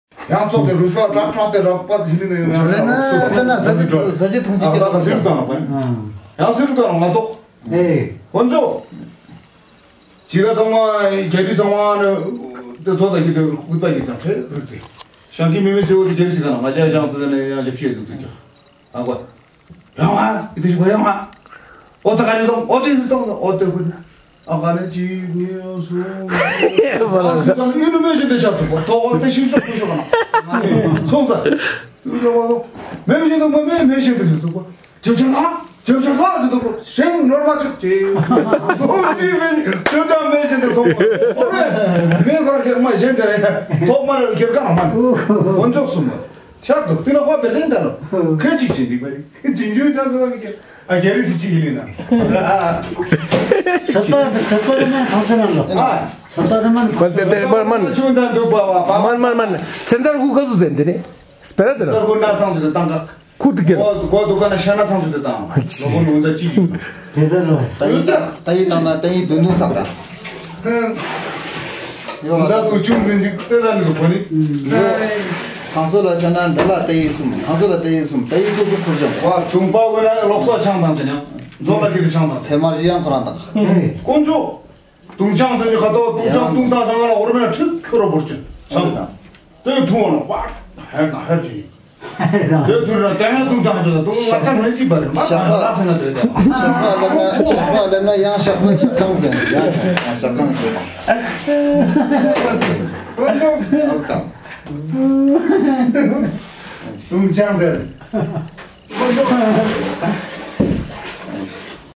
bY Jam. Wangchuk
Mr. Jamyang is joking with the monks at Matho Gonpa, Ladakh, NORTH INDIA